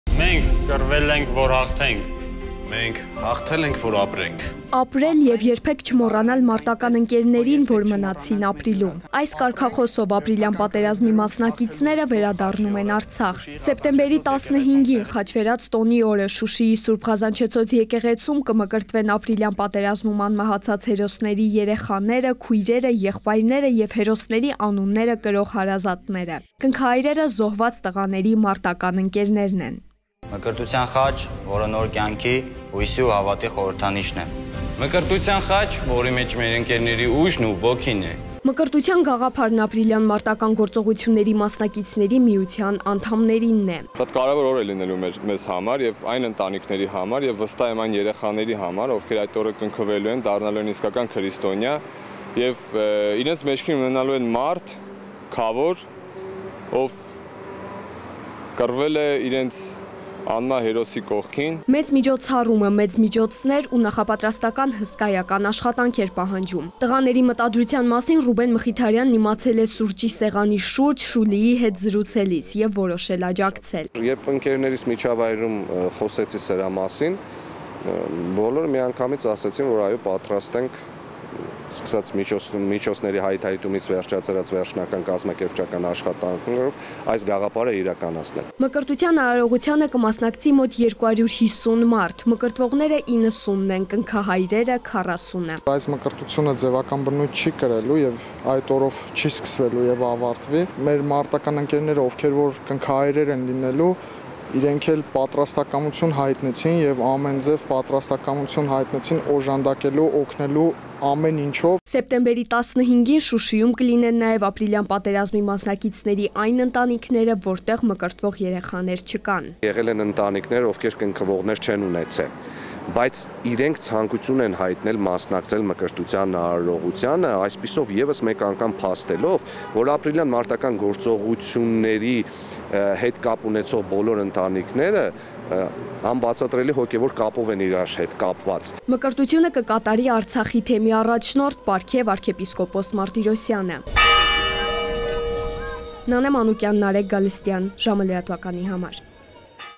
Ապրիլյան պատերազմում զոհվածների հարազատները կմկրտվեն Շուշիում
Սեպտեմբերի 15-ին` Խաչվերաց տոնի օրը, Շուշիի Սուրբ Ղազանչեցոց եկեղեցում կմկրտվեն ապրիլյան պատերազմում անմահացած հերոսների երեխաները, քույրերը, եղբայրները և հերոսների անունները կրող հարազատները: Կնքահայրերը զոհված տղաների մարտական ընկերներն են: